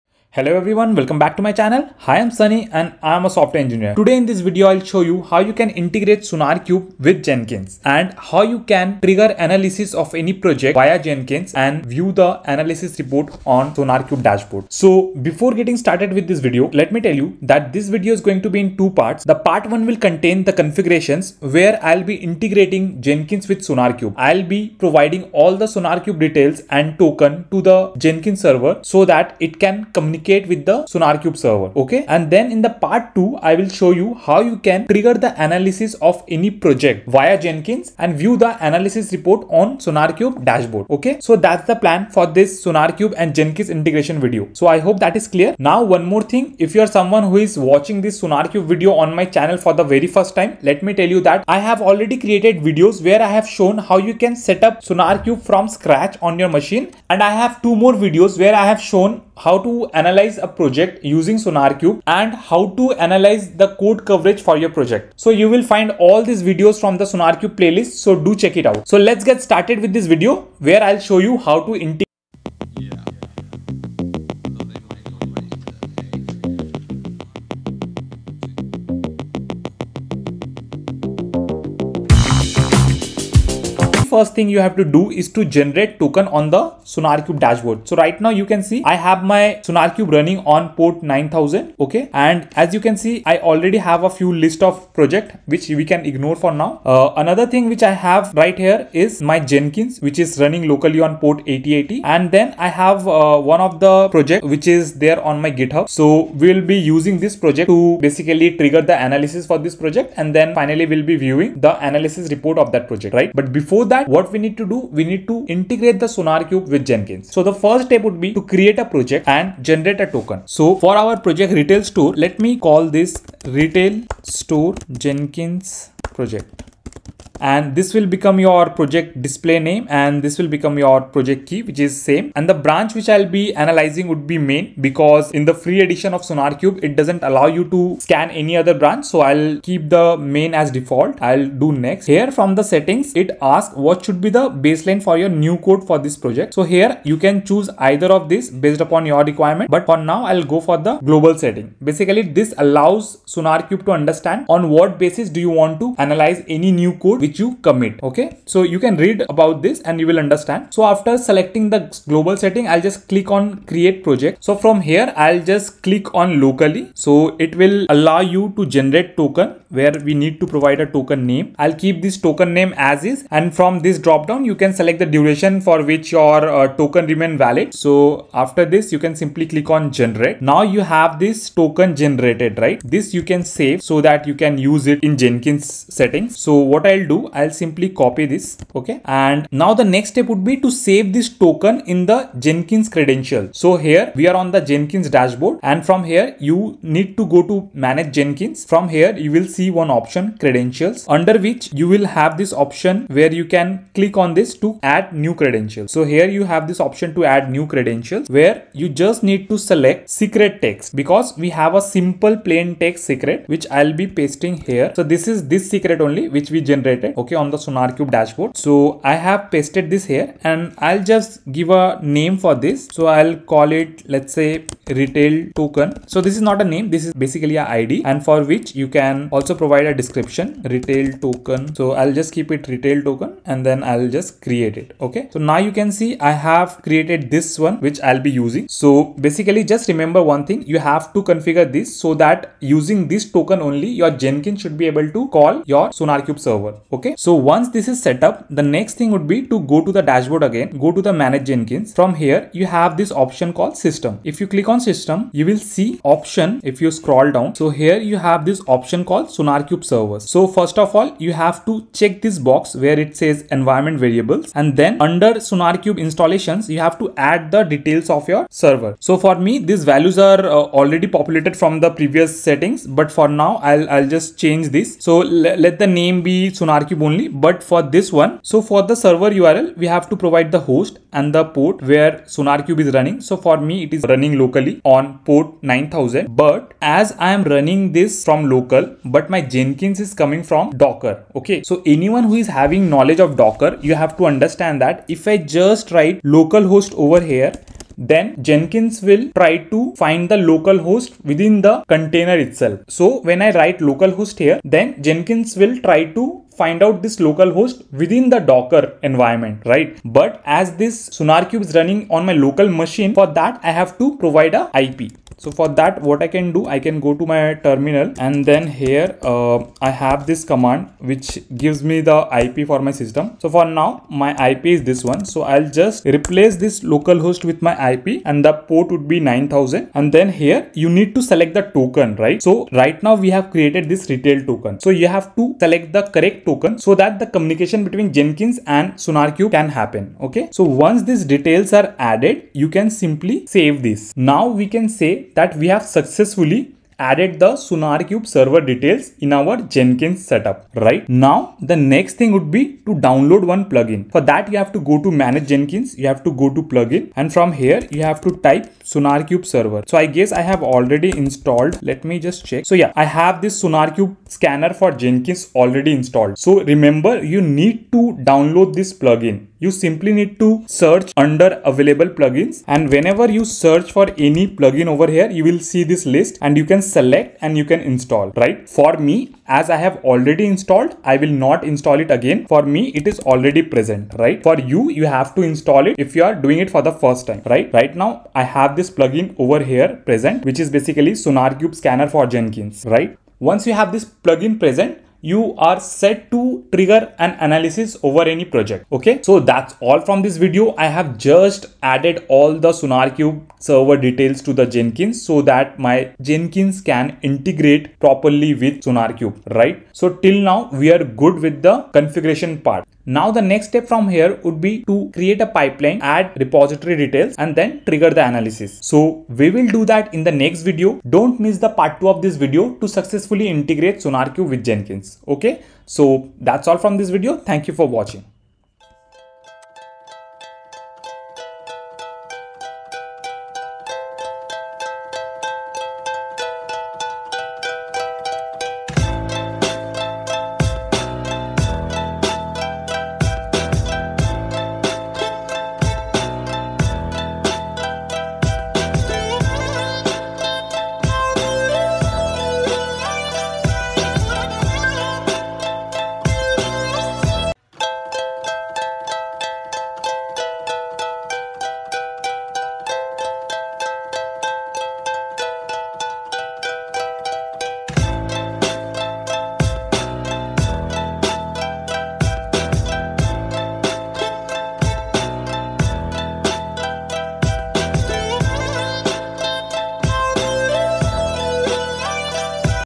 🔥Complete Integration of Sonarqube with Jenkins 🔥 || Part - 1 || engineerhoon
Welcome to Engineerhoon !

Hey everyone! In this video, I'll show you how to connect SonarQube and Jenkins. This will help improve your code and make sure everything runs smoothly. It's a must-know for developers. Let's dive in!

🕒 Timestamps:

0:00 Intro & Agenda
0:46 About SonarQube playlist
01:23 Generate token
03:10 Add token in Jenkins credentials
04:09 Add SonarQube server in Jenkins
5:54 Download SonarQube scanner plugin
06:55 Outro